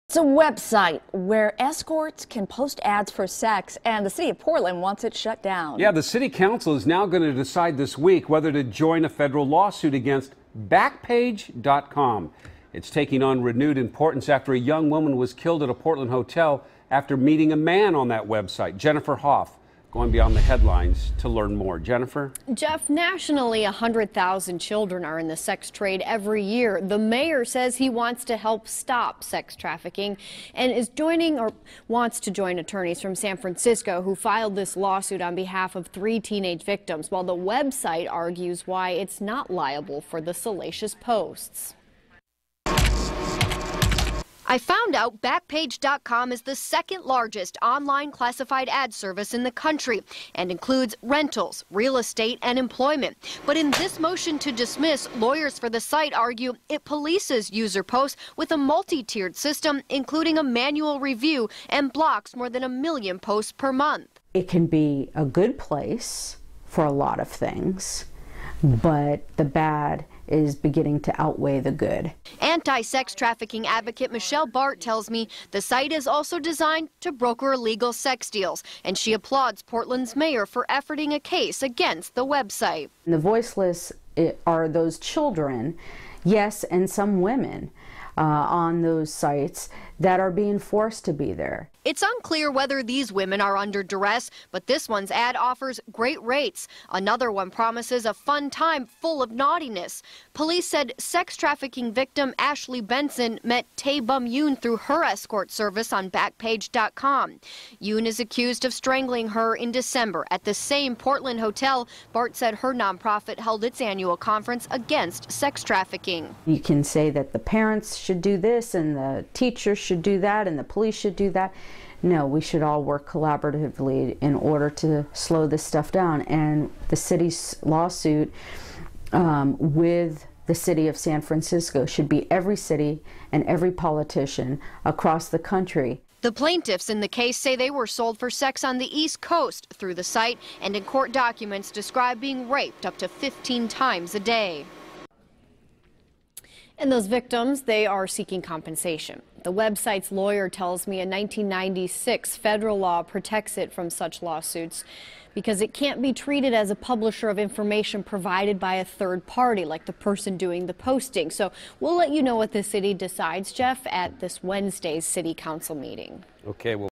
Portland may join suit against sex trafficking site
In the wake of the recent murder of sex trafficking victim Ashley Benson, the city of Portland may join San Francisco in filing a friend of the court brief in a case against Backpage.com.